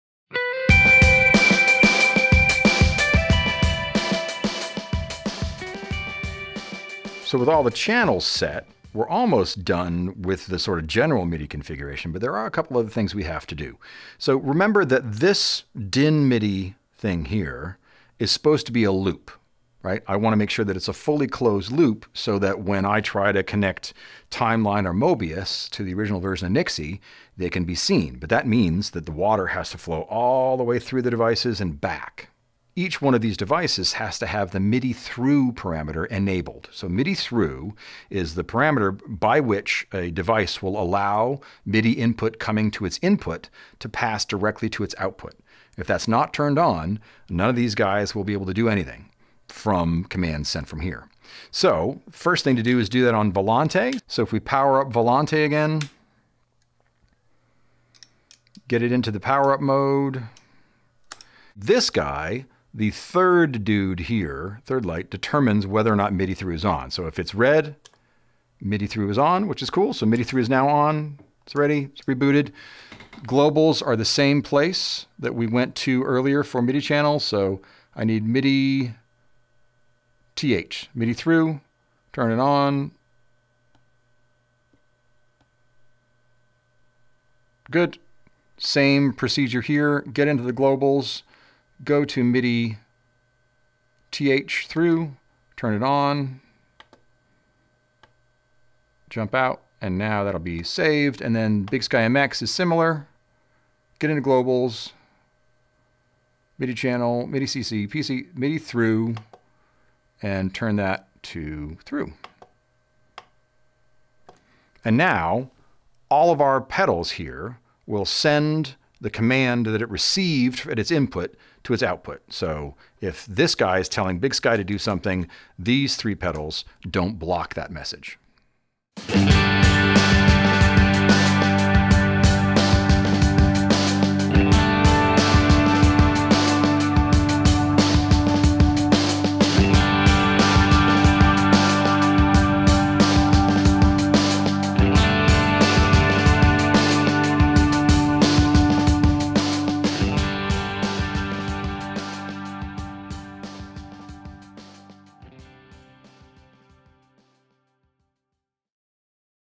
Strymon Tech Corner: Setting MIDI Thru On Strymon Pedals
Join Sean as he demonstrates how to configure the large format Strymon pedals on his board to enable MIDI Thru. MIDI Thru allows MIDI data received at a device's MIDI input to be passed directly to its MIDI output, which is essential for allowing full communication in a closed MIDI loop.

On this board, Compadre, Ultraviolet and Brig are connected to MIDI via a Strymon Conduit pedalboard MIDI interface, which you can learn more about